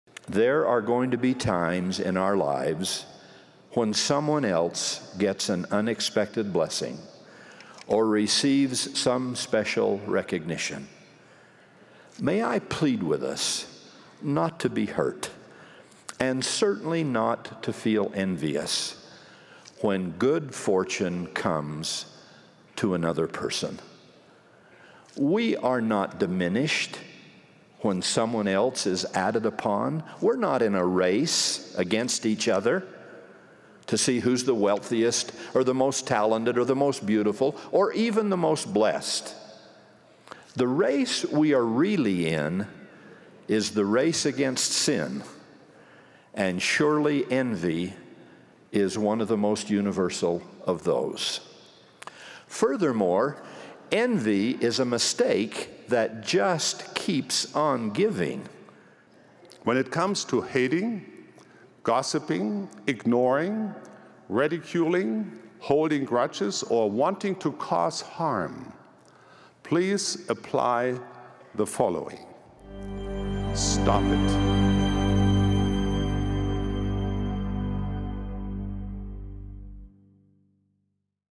Stop IT!!!
A Universal message Just for YOU!
The practice of comparing yourself to others is usually at the root of envy. It causes you to feel you're not good enough and that in order to be acceptable you have to achieve more.
Envy begins with one’s feelings about oneself. Nobody can bring you peace but yourself.
When you make peace with yourself, you'll make peace with the world!